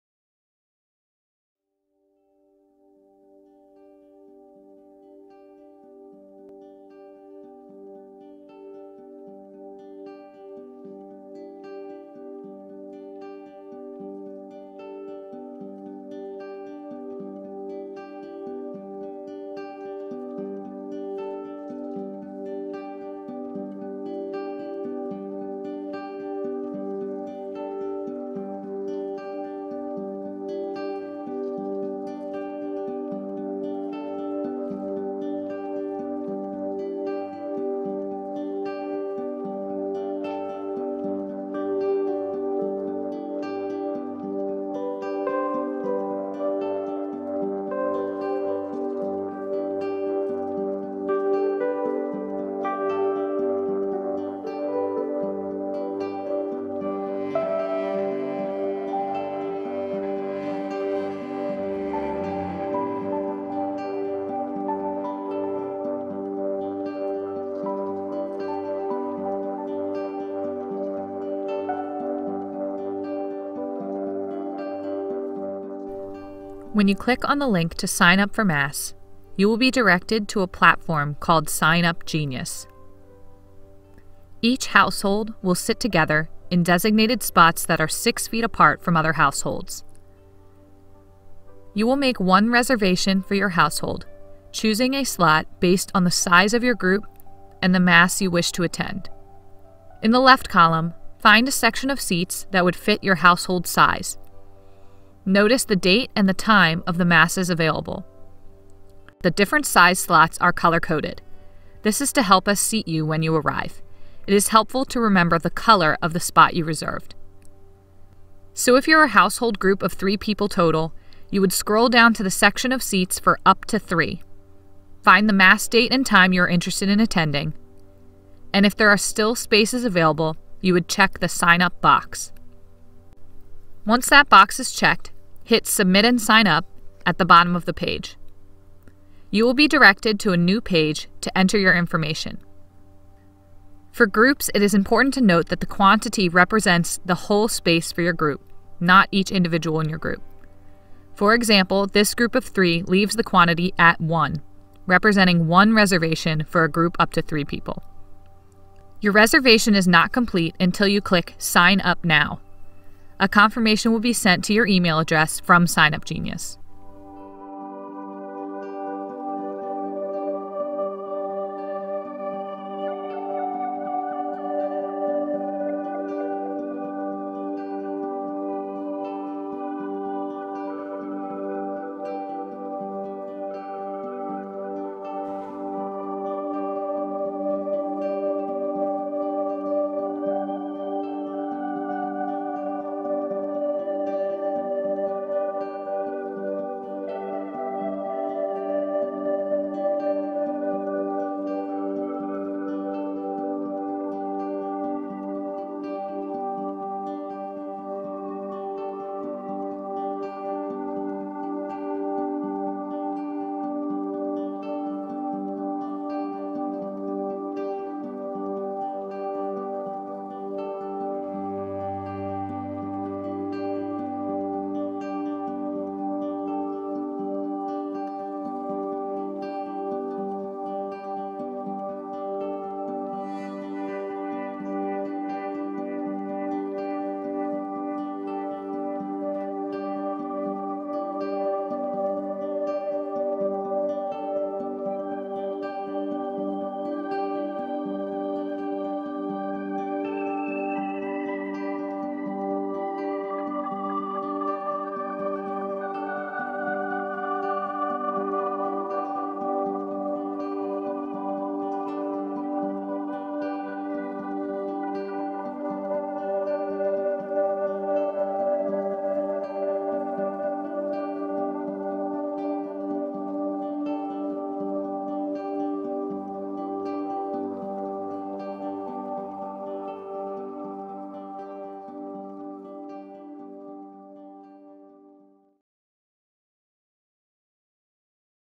Information about Reopening Masses at Mother Seton
Link to sign up for daily Mass only is available now on MotherSetonParish.org. When we are able to open up for Sunday Masses our full sign-up will be available.

For now, daily Mass sign-ups are simply by number of people attending.

Please contact our office if you have any questions.